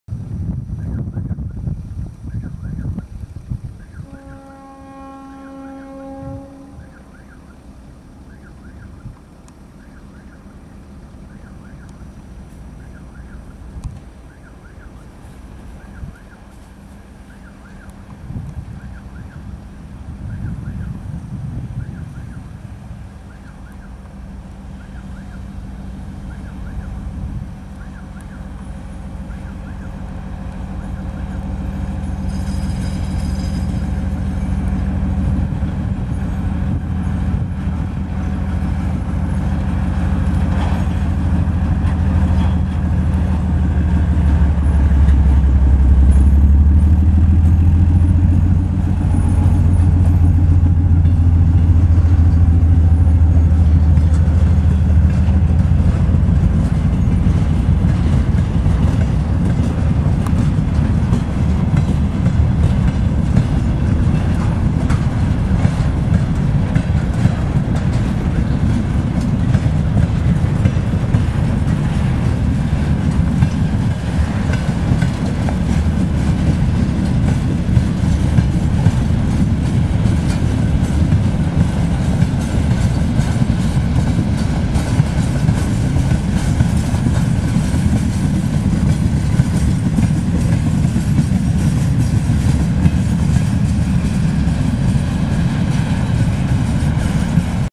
Тепловоз М62-1227 прибывает на ст. Краслава / M62-1227 arriving to Kraslava station
M62-1227 arriving to Kraslava station, Latvia, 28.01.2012